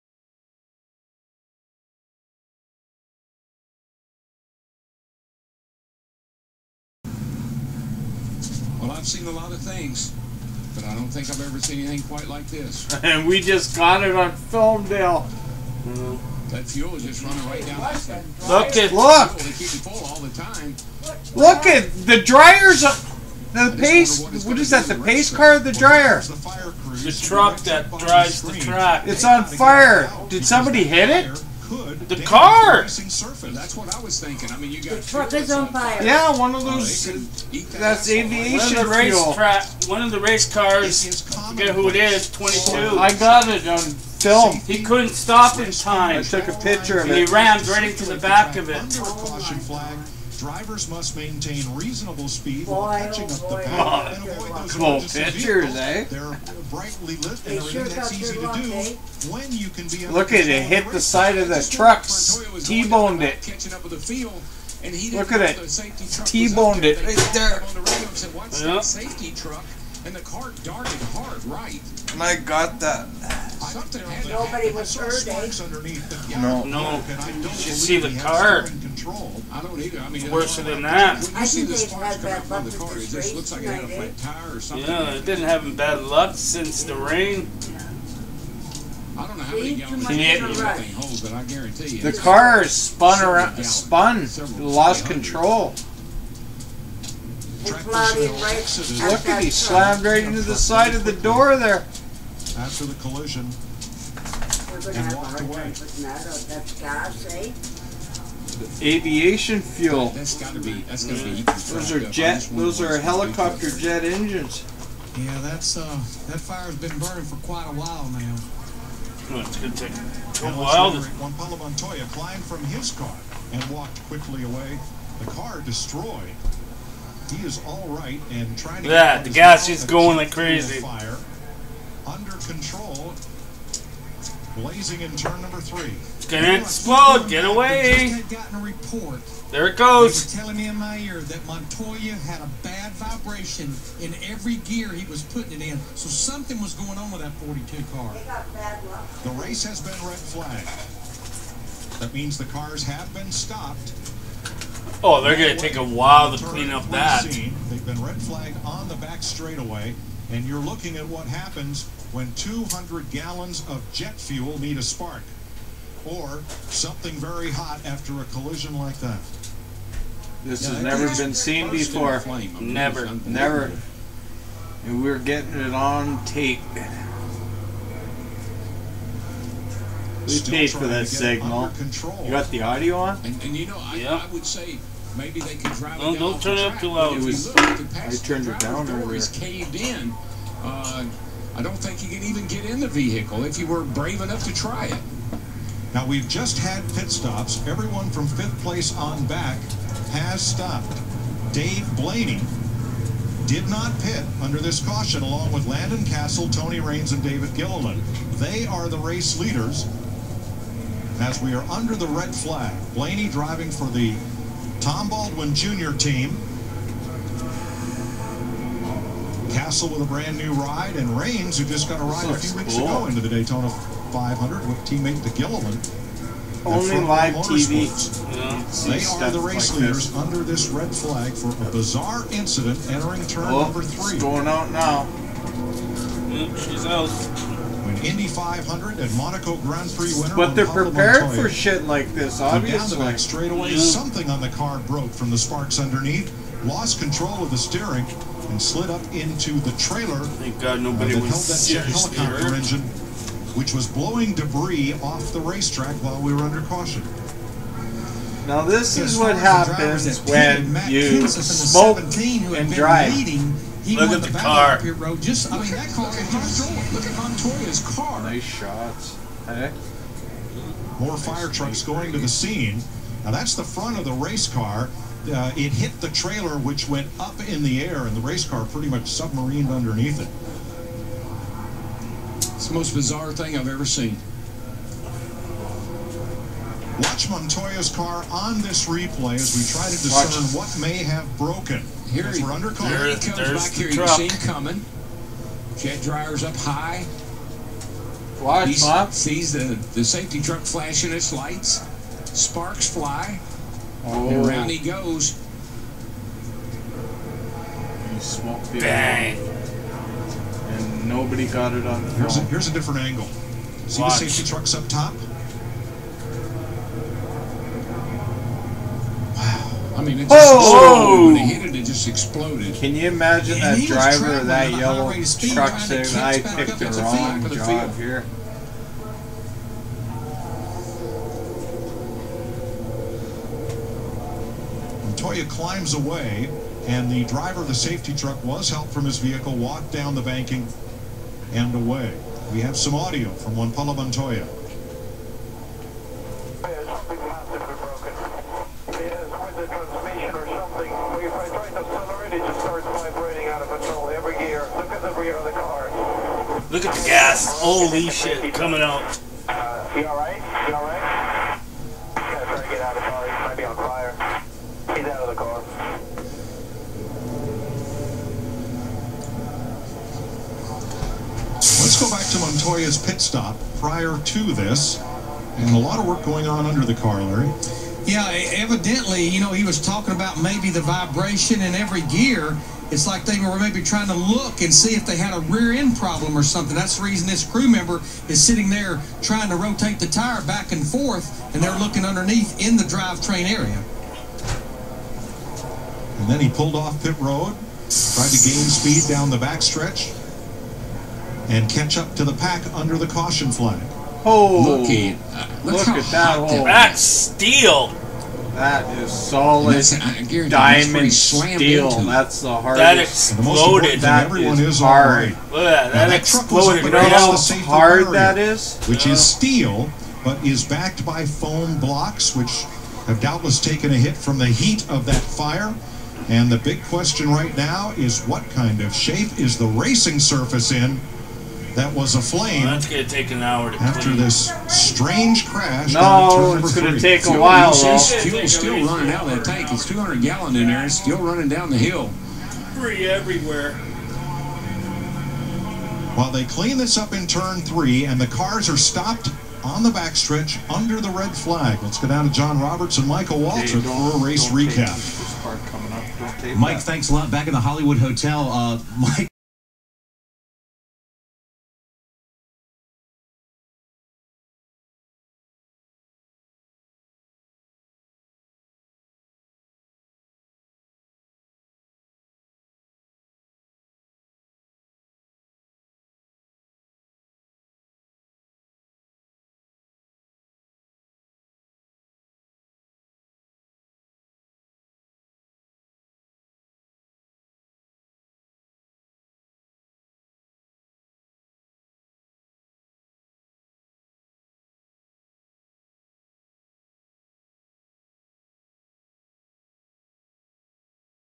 the full watch on the jet dryer crash of the night
#42 hit the the dryer truck that drys the track no one was hurt and the clean up crew took all most 2 hours to clean up the mess a race that lasted in two days feb 27 to the 28th 2012